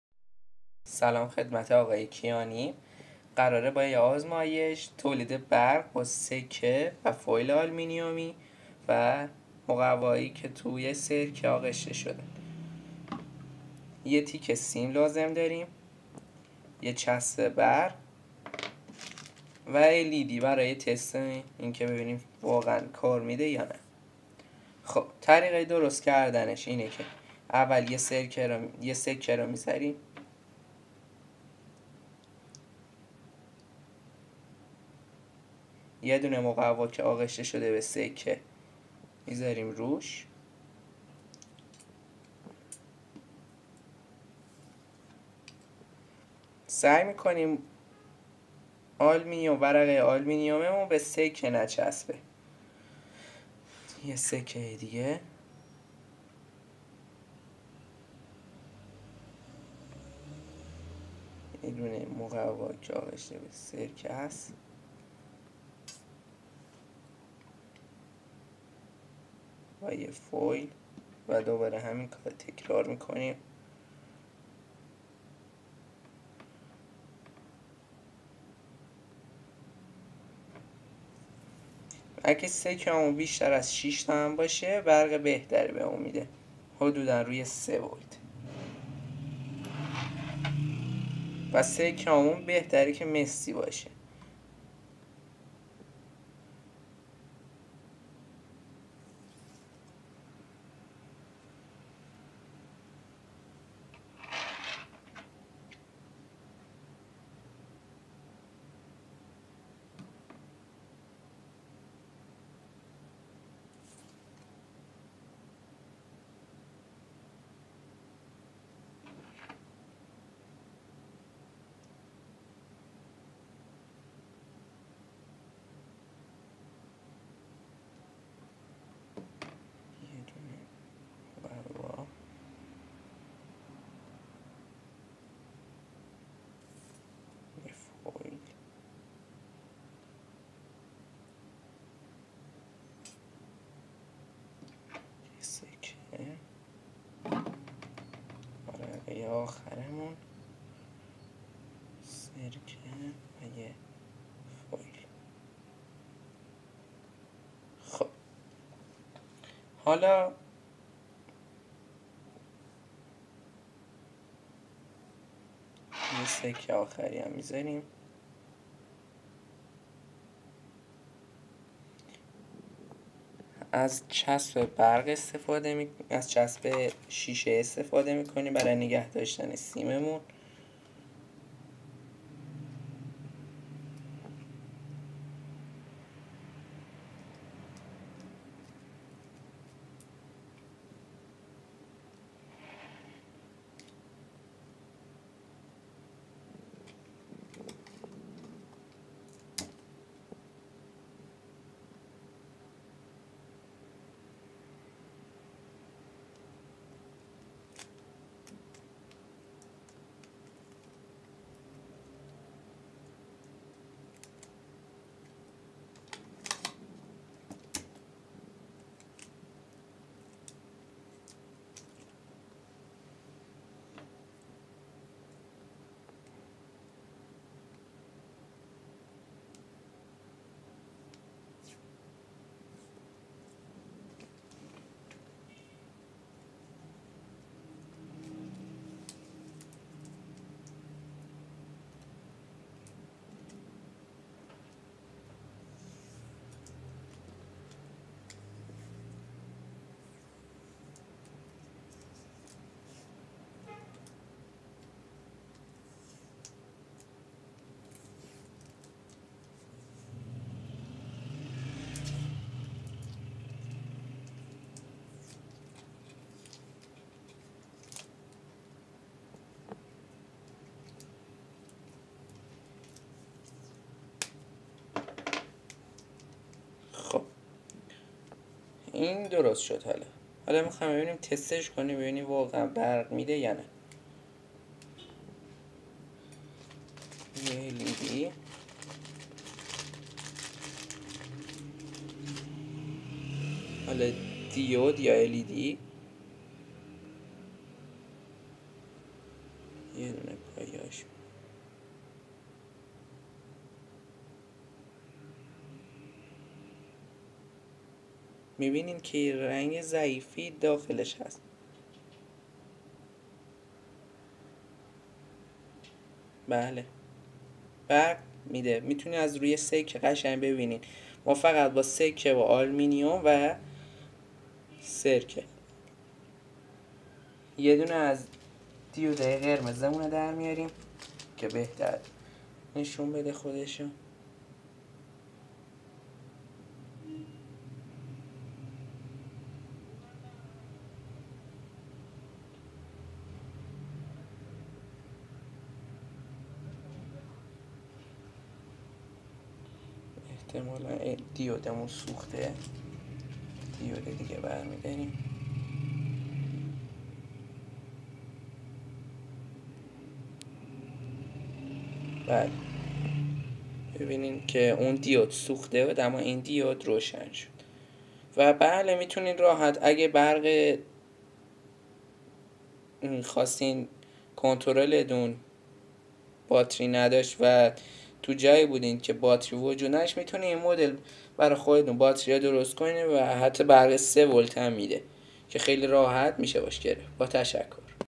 battery
ساخت باتری با سکه و فویل آلومینیم و سرکه و مقوی

اجرا و ساخت کلیپ سید محمد رستگار